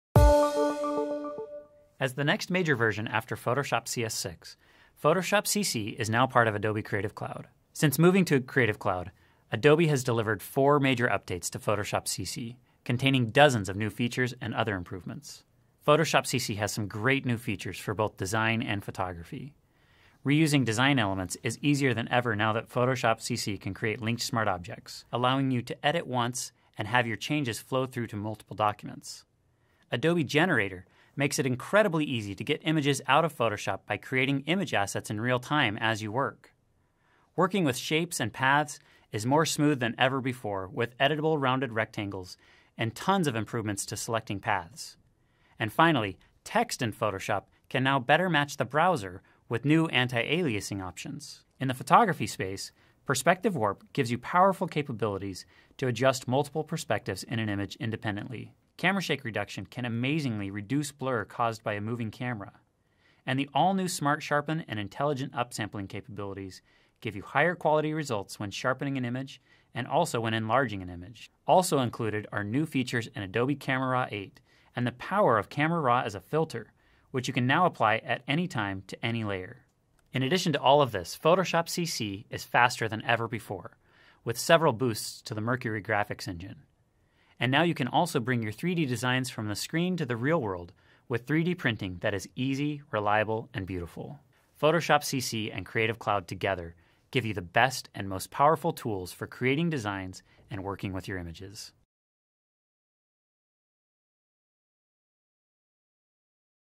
Adobe Systems Photoshop CC Overview link in description Photoshop Lightroom for US $9.99/mo
Photoshop Product Manager, Stephen Nielson, shares a quick overview of a handful of the exciting features in Photoshop CC including Camera Shake Reduction, Perspective Warp, linked Smart Objects, 3D printing, Adobe Generator, and more.